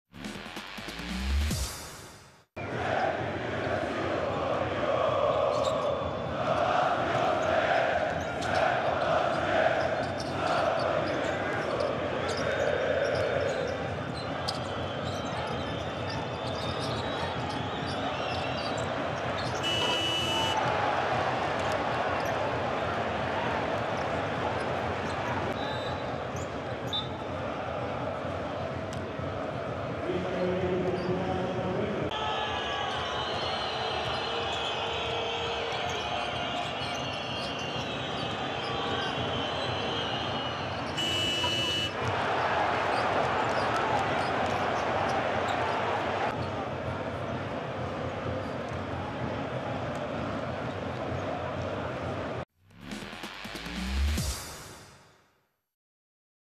Facu hits two CRAZY 3-point shots in the 1st half (Partizan Mozzart Bet - Crvena zvezda Meridianbet)
Well, check out these two 3-point shots made by Crvena zvezda Meridianbet star player Facundo Campazzo.